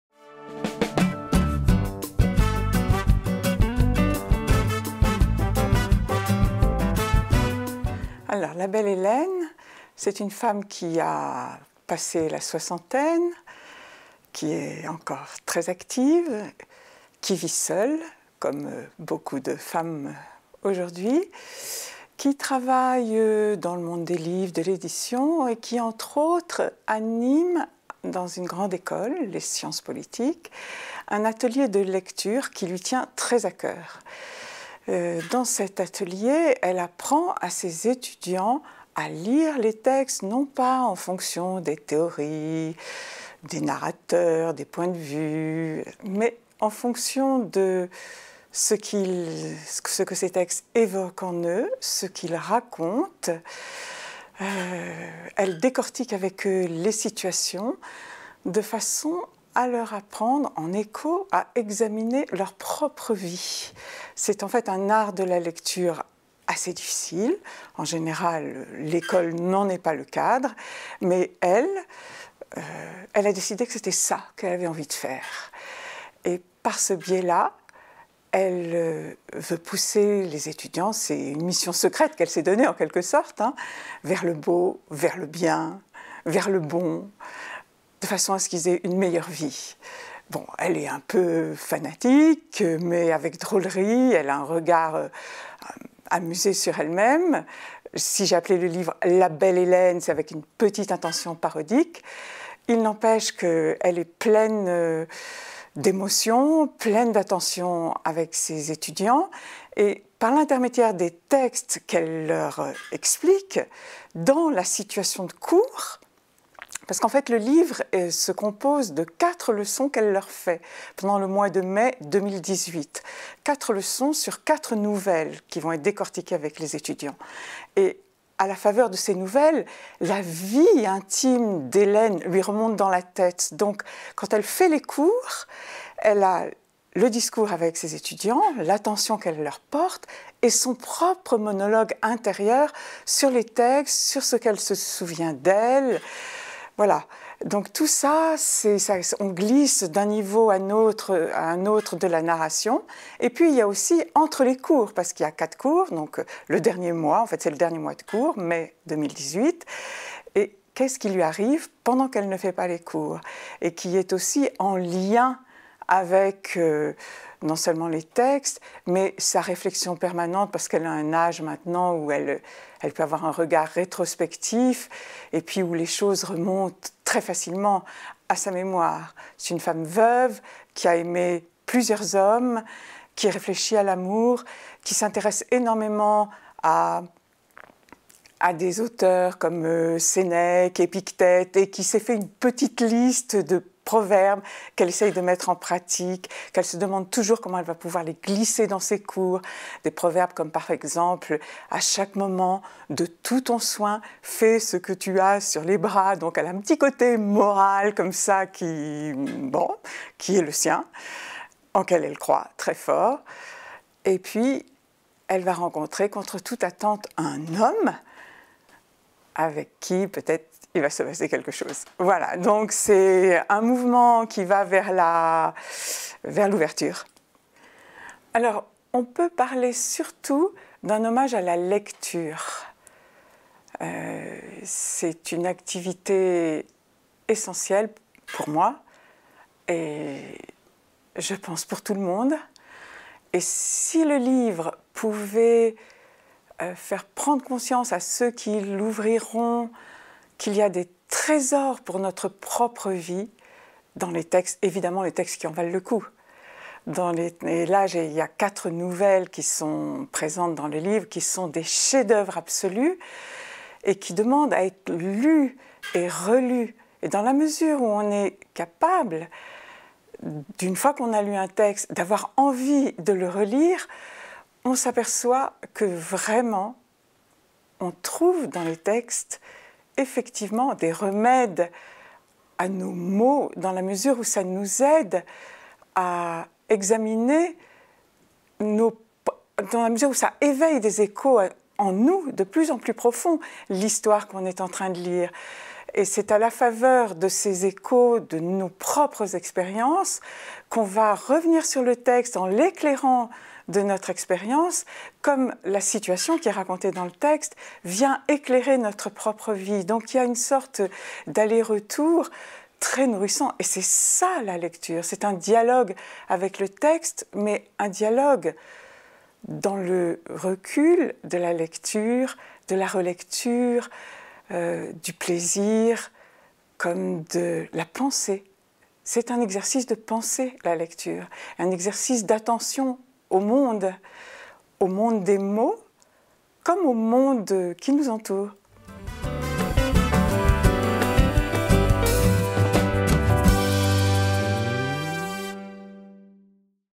Pascale Roze - La belle Hélène
Pascale Roze nous présente son livre " La Belle Hélène" aux éditions Stock. Rentrée littéraire janvier 2020

Catégorie
Actualités et politique
Licence